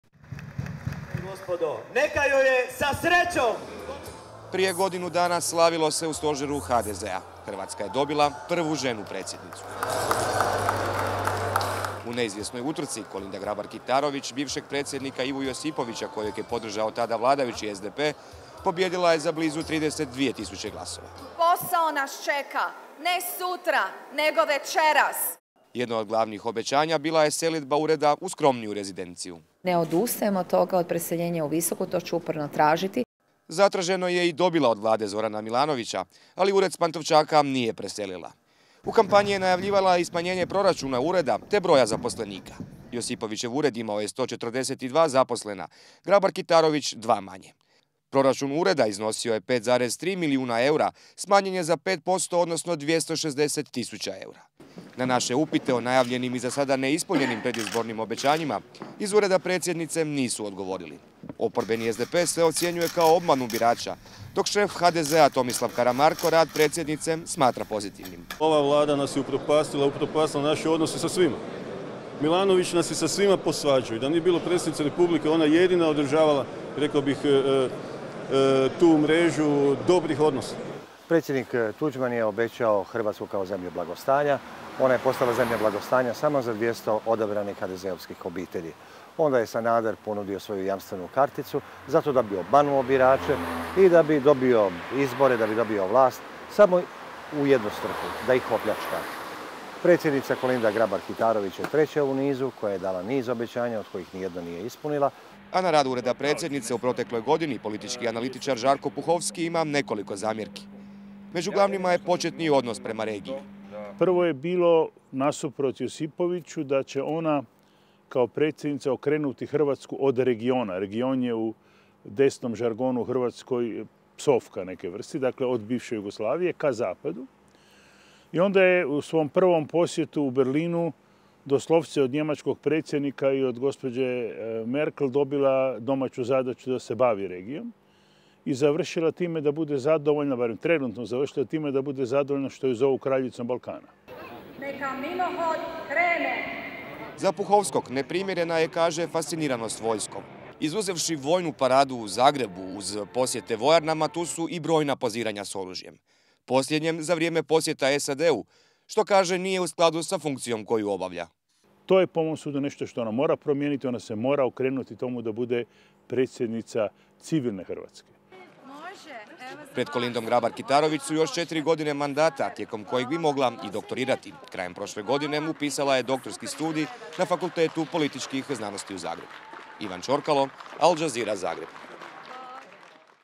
Godina mandata Kolinde Grabar-Kitarović
Sutra se navršava godina dana od izbora Kolinde Grabar-Kitarović za hrvatsku predsjednicu. Kao kandidatkinja HDZ-a i partnera pobijedila je Ivu Josipovića u drugom krugu. No, od izbora do danas, neka od glavnih obećanja s kojima je odnijela pobjedu i prikupila više od milion i sto hiljada glasova, ipak nije ostvarila.